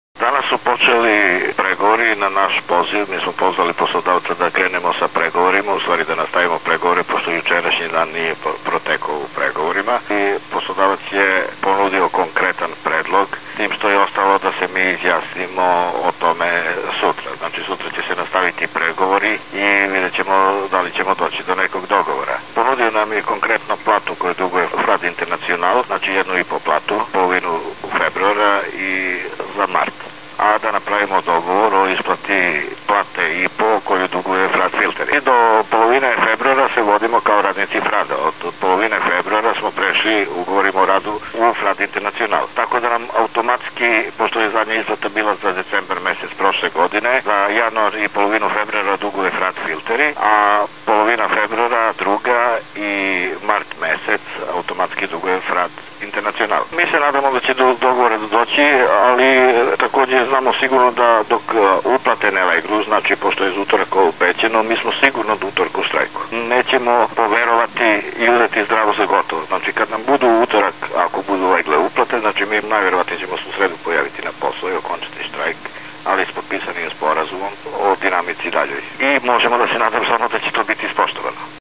Na pomolu dogovor štrajkača i uprave Frada?
Govori predsednik samostalnog sindikata Mlađan Marković